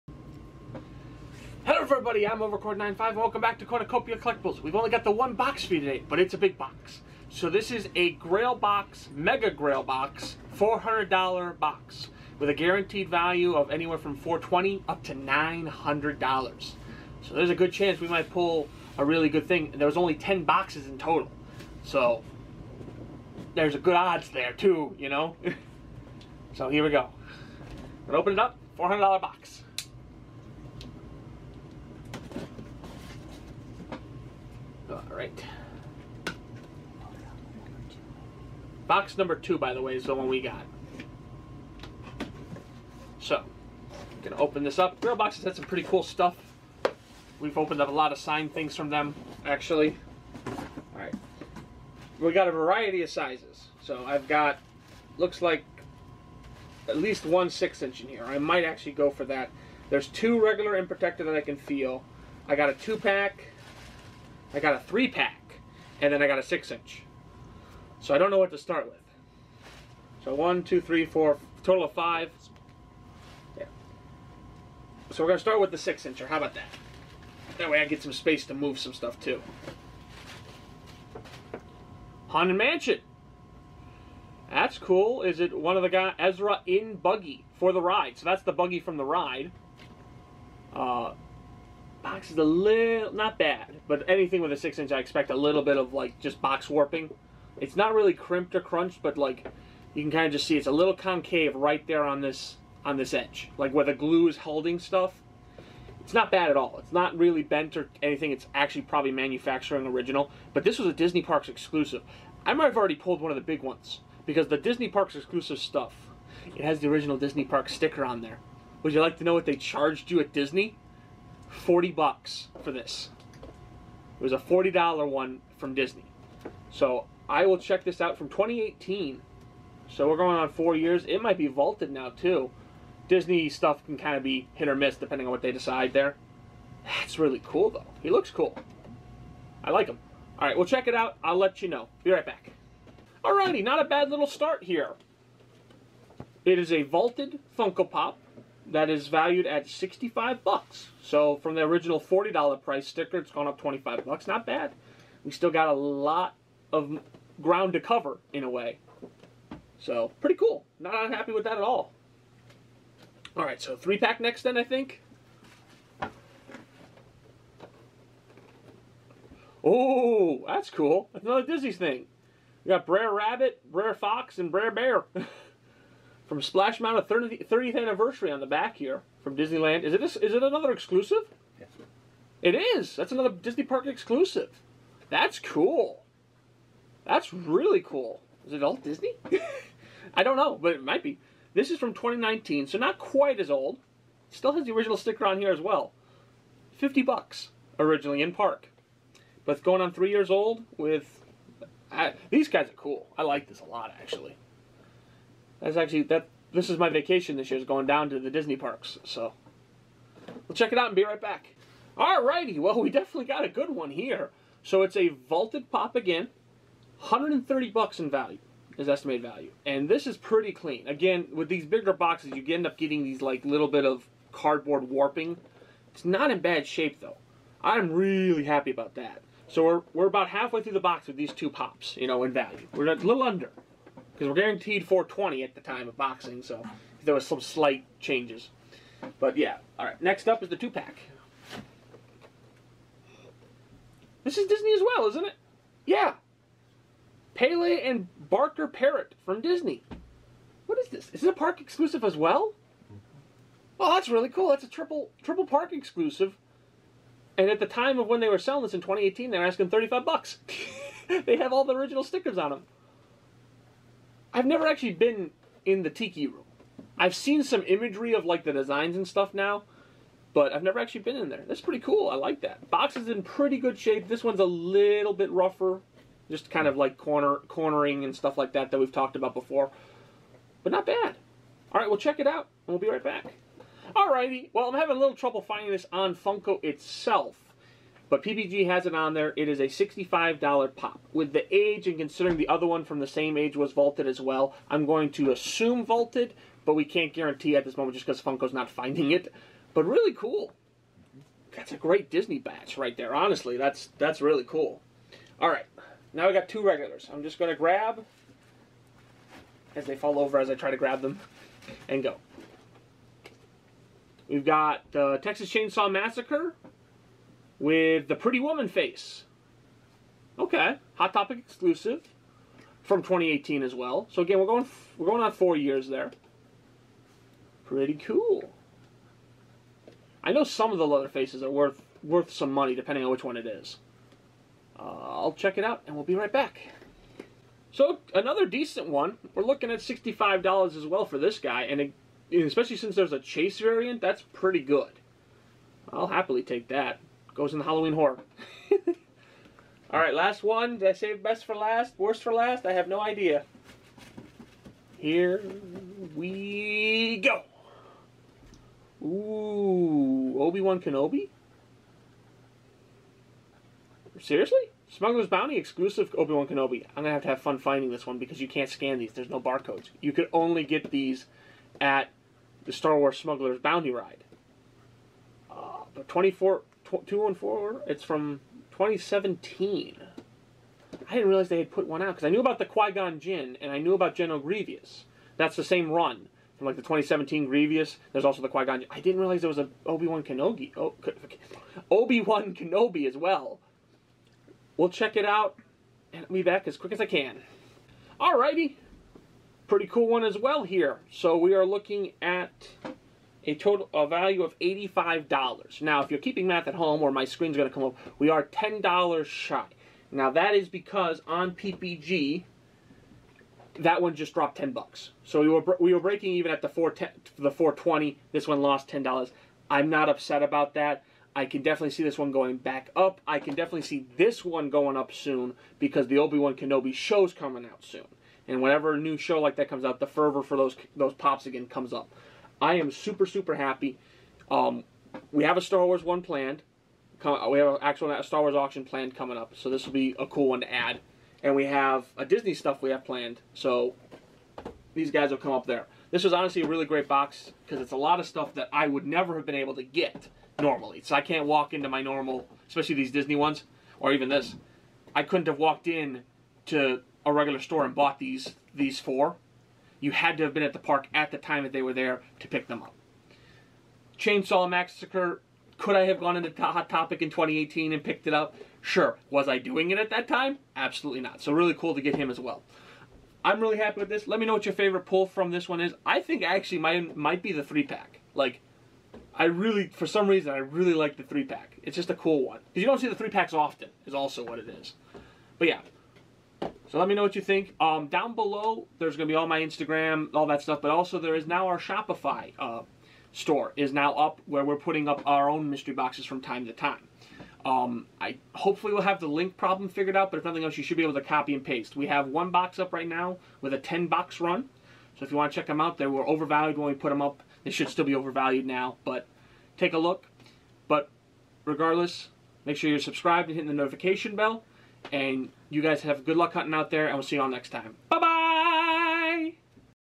$400 Grail Box Funko Mystery Box. Multiple Disney Goodies inside
A really Cool Box from GrailBox with some awesome Disney Pulls!

Check out the Whatnot app. Sign up with this link and you will get $10 off your first purchase.